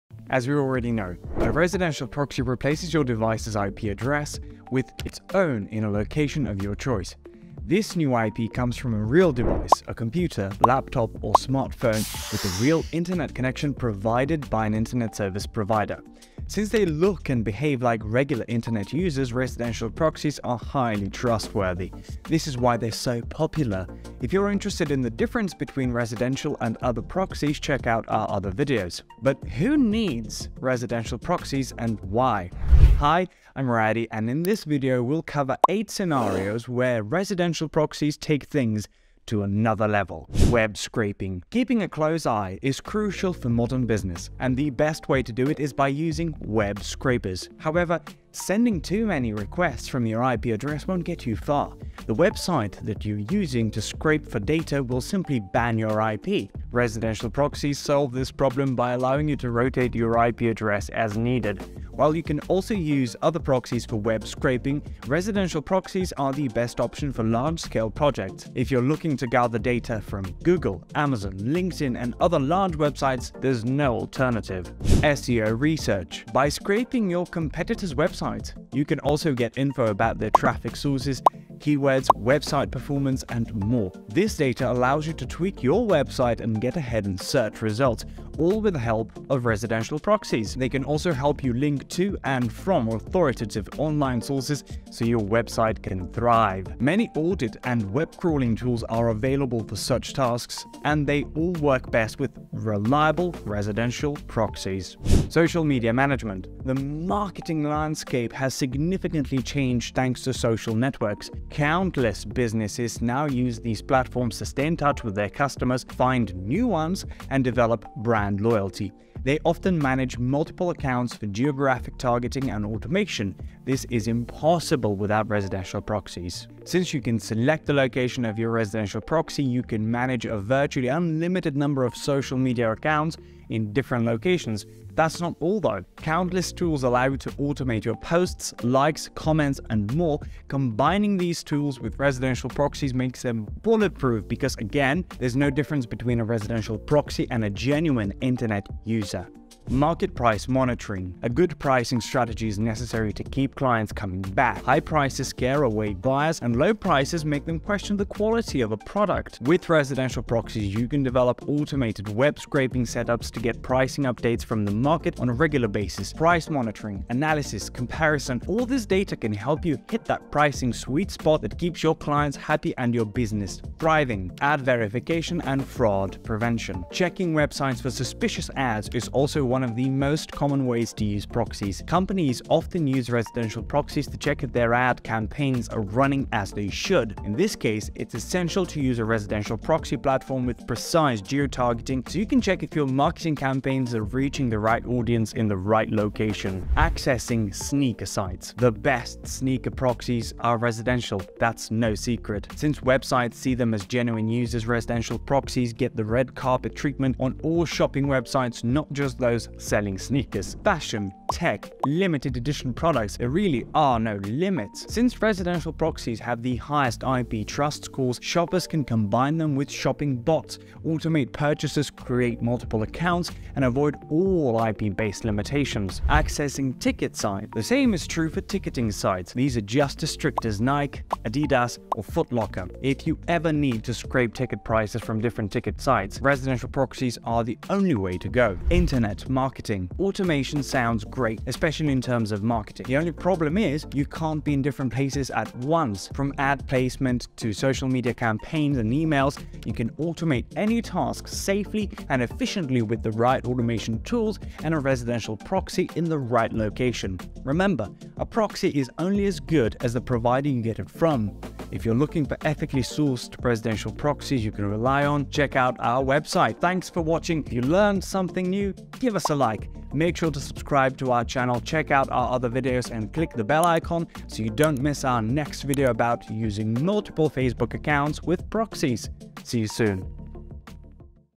Residential Proxies Explained | Top 8 Ways to Use Them
Residential Proxy replaces your device’s IP address with its own in a location of your choice. This new IP comes from a real device - a computer, laptop, or smartphone - with a real internet connection provided by an internet service provider.

Since they look and behave like regular internet users, residential proxies are highly trustworthy. This is why they’re so popular.

In this video, we’ll cover eight scenarios where residential proxies take things to another level!

Choosing the right proxy option can be overwhelming, given the wide variety available today. You want a solution that offers robust privacy protection while still being easy to set up and use. Plus, you want to ensure you’re getting excellent value for your money.

If you’re new to the world of internet proxies, you may assume that all proxies are the same. However, different types of proxies are better suited for different scenarios.

Residential proxies are the clear winners when it comes to anonymity and undetectability because they use IP addresses obtained from actual ISP customers. In contrast, datacenter proxies use IP addresses from server farms, making them easier to identify and block. The type of IP address used can be crucial for specific tasks such as video streaming, online shopping, and banking, which have extra security measures to detect and block proxies.

ISP-sourced residential proxies overcome these limitations because they appear as regular customer traffic to servers, making them undetectable. With the growing popularity of streaming services, unrestricted access to content is increasingly important.

However, streaming sites have ramped up efforts to detect and prevent the use of proxies, leading to user complaints and issues in recent years.

0:00 Residential Proxies Working Principle
0:40 Web Scraping
1:15 SEO Research
1:45 Social Media Management
2:29 Market Price Monitoring
2:56 Add Verification & Fraud Prevention
3:17 Sneaker Sites
3:47 Accessing Ticket Sites
3:59 Internet Marketing
4:23 Iproyal Residential Proxies
4:35 Thanks for Watching